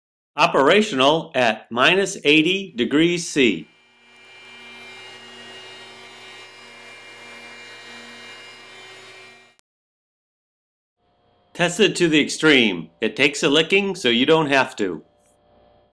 Extremely Cold
Shows unit operational with -80C Fluid